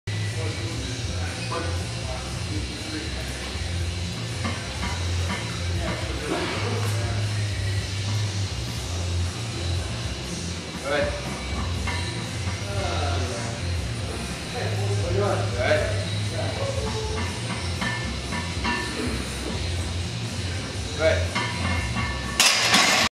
Shirt Bench W1 111710
3x265 (84%).  First time in 44 Katana; wasn't able to touch my top set but kept a solid bar path.